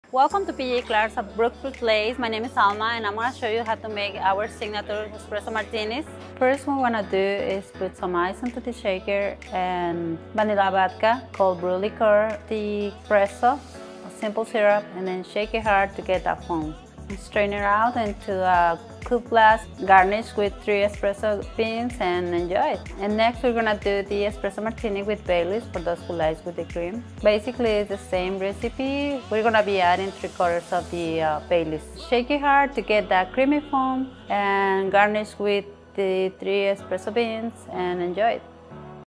P.J. Clarke's Espresso Martini at BrookfieldPlace
Bartender Alma at PJClarkes shows us how to make two versions of their popular EspressoMartini. 🍸To make your own add ice to a shaker, followed by vanilla vodka, cold brew liqueur, espresso shot, simple syrup, and shake it vigorously for that creamy foam. Serve in a coupe glass and garnish with three espresso beans. ☕ What's your go-to P.J. Clarke's cocktail?